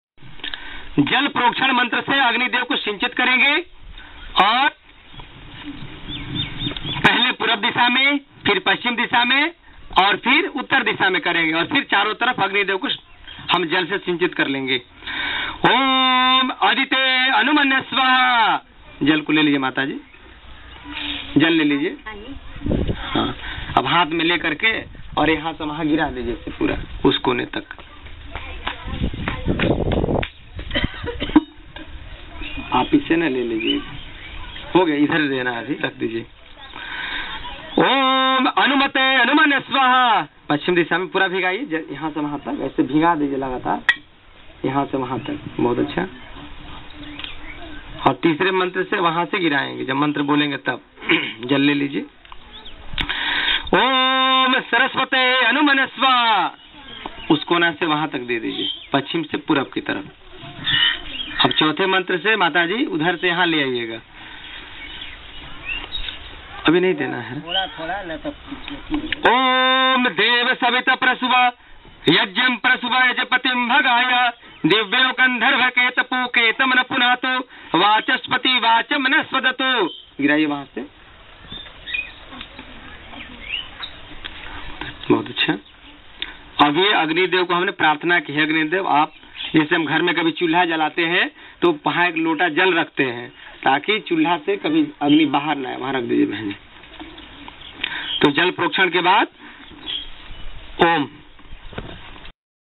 Yogi Dr.Brij Mohan-Yog Pracharak|Yog Shivir At Jaynagar|Bhojpur|Bihar|24-03-2017
DSCN5305
04-State-BIHAR,District-BHOJPUR,Month-March-2017,Camp No.-04,Time-6.30AM TO 9.00AM,Yog Shivir-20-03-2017 To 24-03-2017,Vill-Jaynagar,Post-Pianiya,Block-Udwantnagar,District-Ara(Bhojpur),State-Bihar,Video Cliping Date-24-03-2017